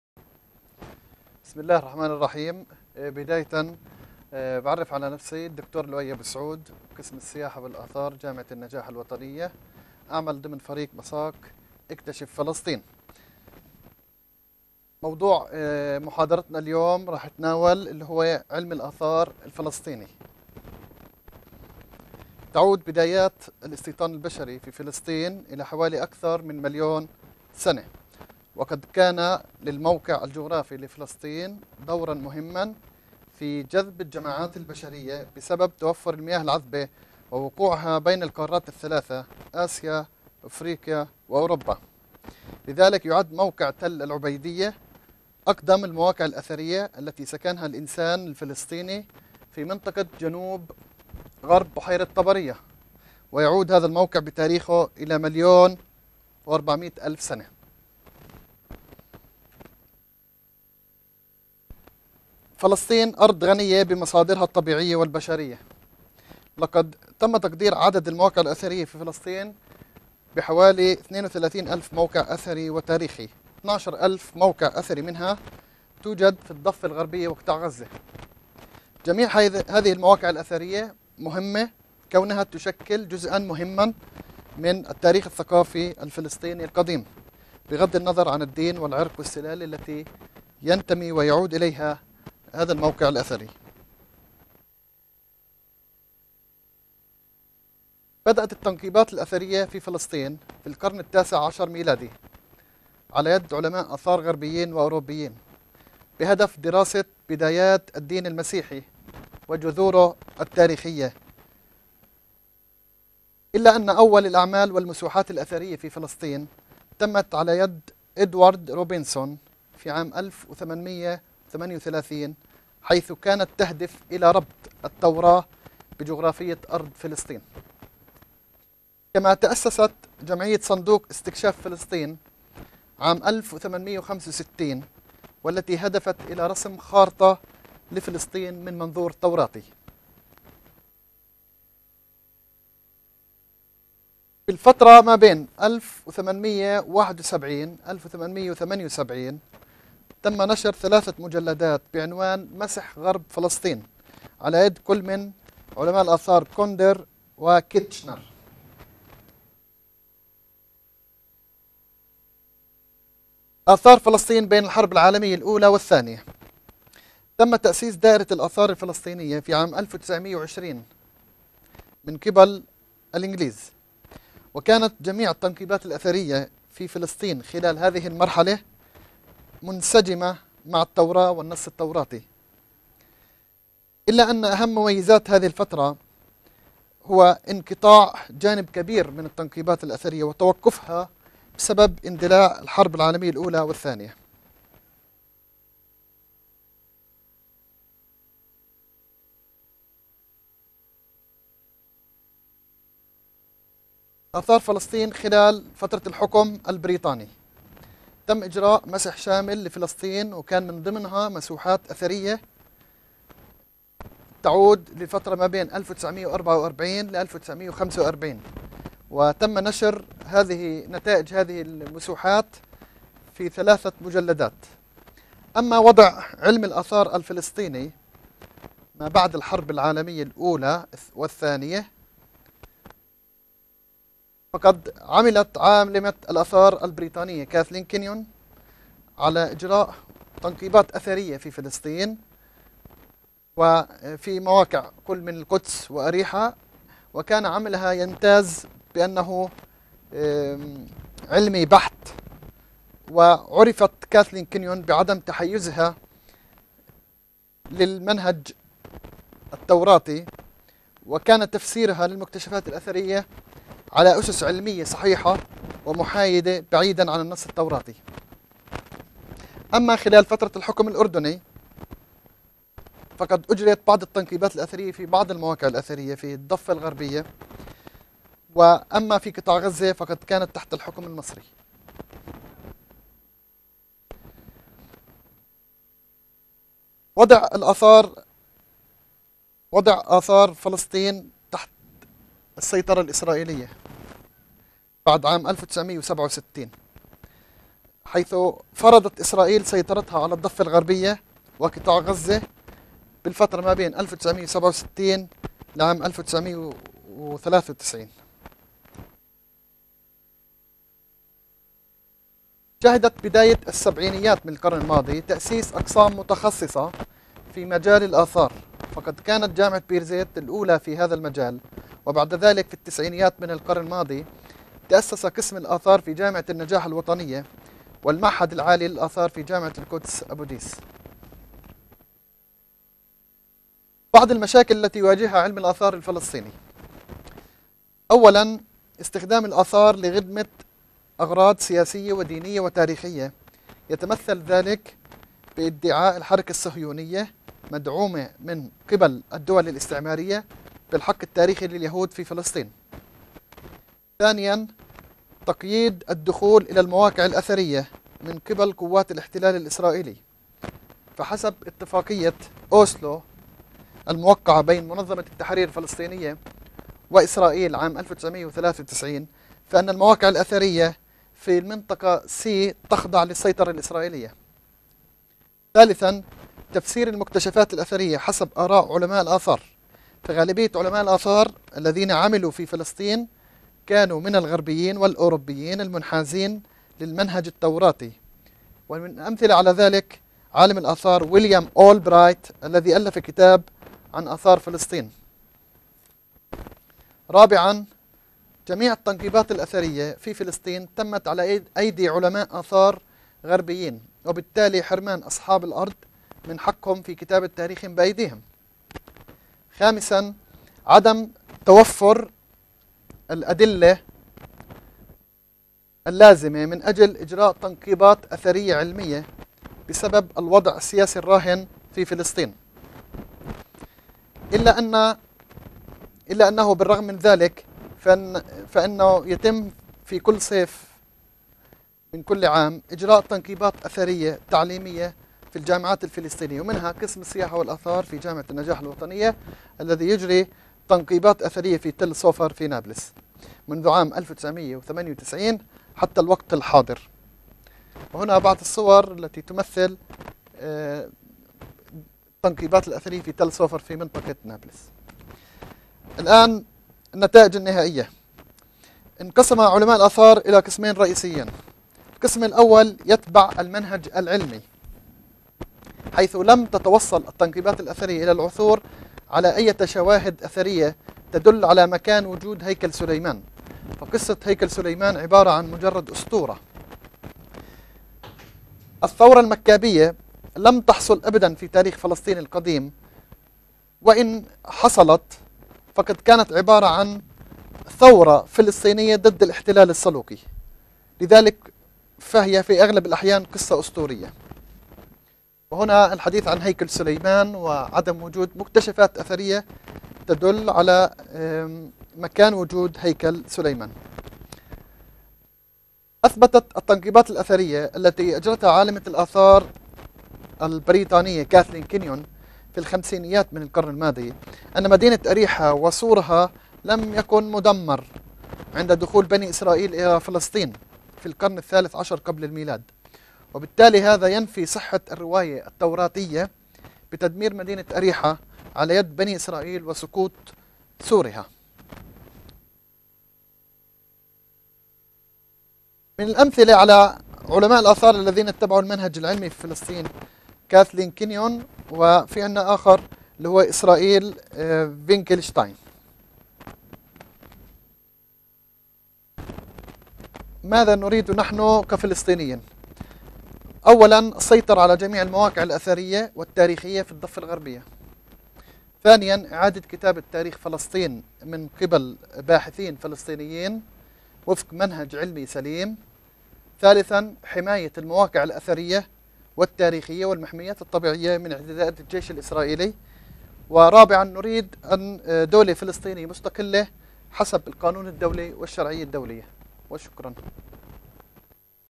Archaeology In Palestine
Presentation about archaeology in palestine
By dr. Loay Abu Soud
An Najah National University
Discover Palestine Course